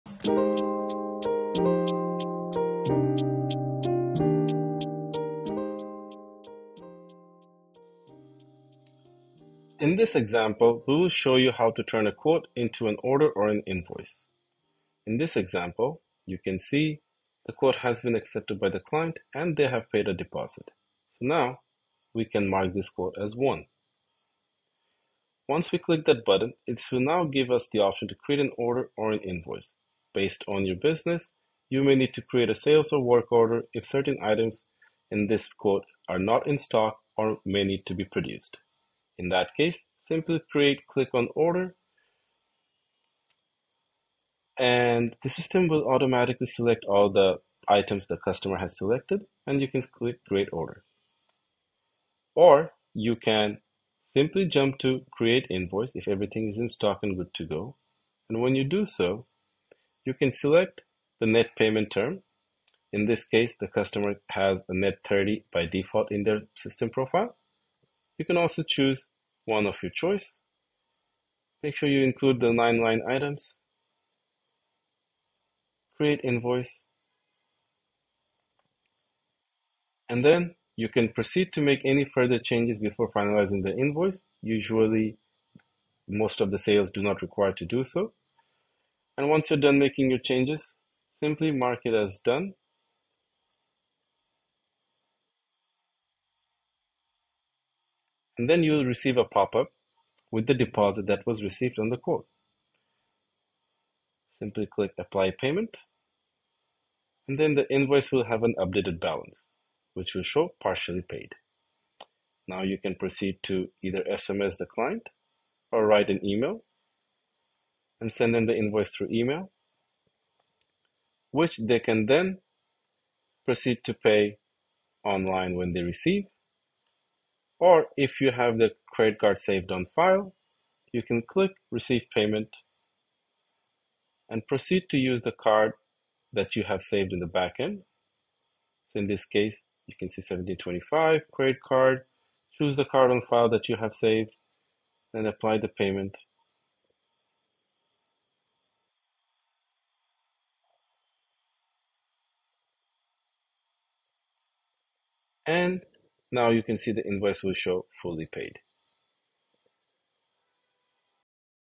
How to transform a quote into an invoice in 2 minutes with QuoteMachine
In this quick tutorial, learn how to convert a quote into an invoice in less than 2 minutes.

If you're interested in learning how QuoteMachine can streamline your processes don't hesitate to book a free demo with one of our specialist